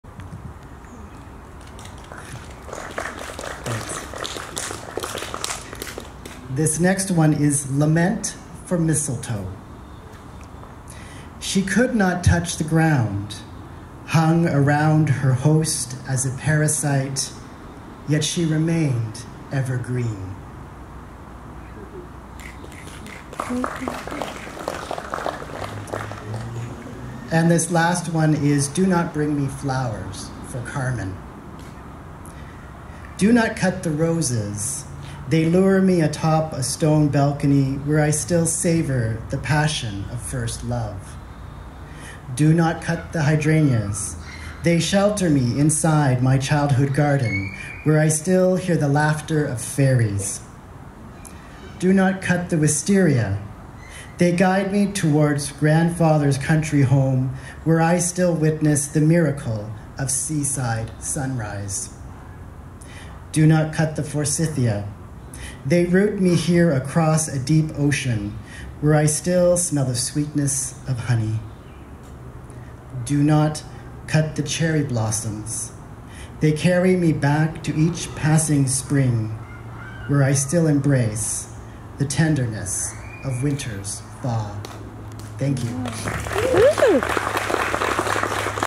Poetry in the Park 2024 - Ryan Gibbs on Open Mic, August 14
August 14, 2024 at Queen's Park, New Westminster
New West Pride Night

Videographer Janet Kvammen
Presented by Royal City Literary Arts Society